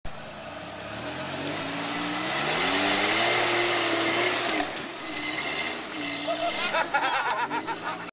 Toyota pickup hill climb
Toyota pickup climbing short steep hill